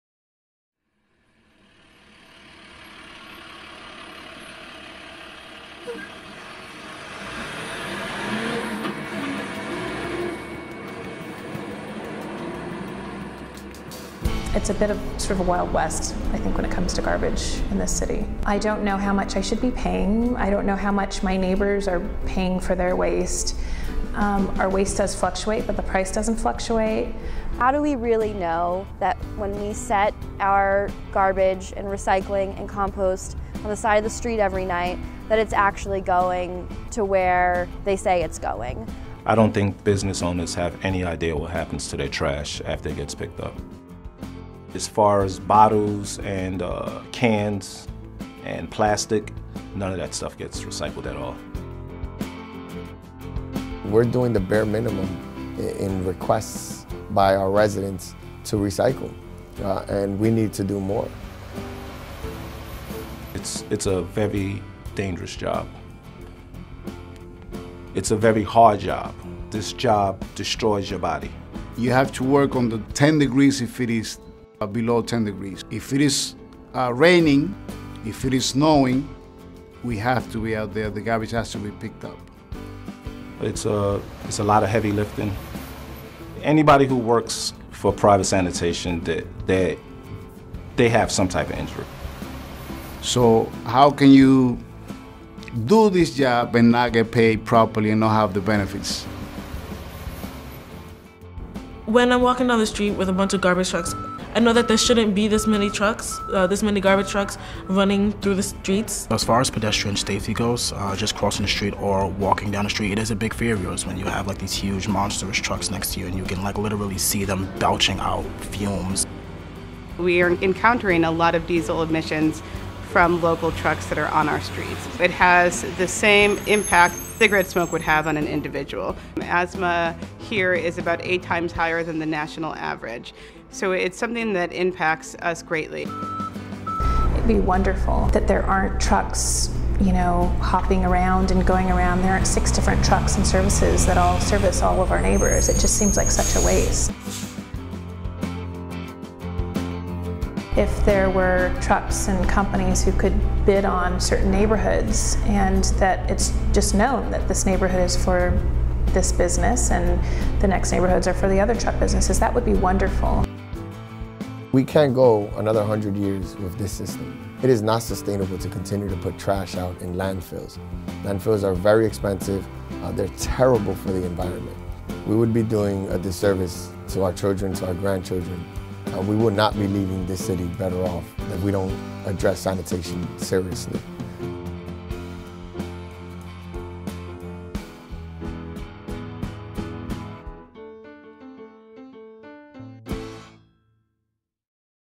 Transform Dont Trash NYC
This powerful video features City Council Member and Sanitation Committee Chair Antonio Reynoso, small business owners, private sanitation workers and young people and advocates from environmental justice communities discussing the negative impacts that the city’s outdated waste system has on them, as well as solutions to create good jobs and clean and safe communities for all New Yorkers.